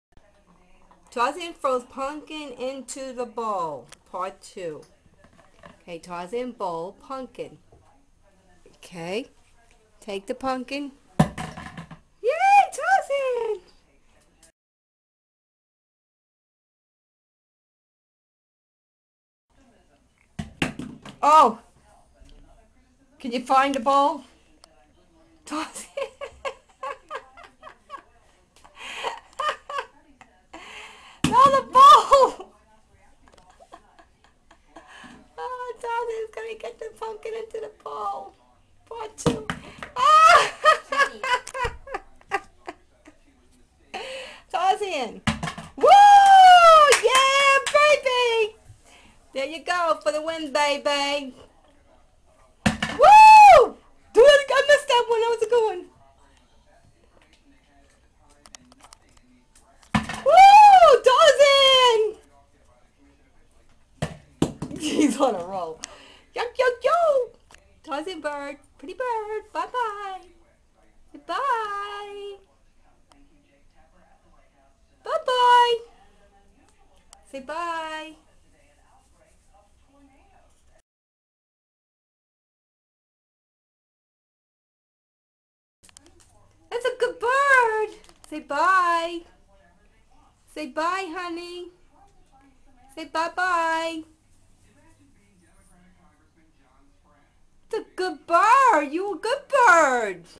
Parrot scores big with pumpkins
tarzan is to funny here we got him to throw the pumpkins into the halloween candy bowl .Happy halloween from tarzan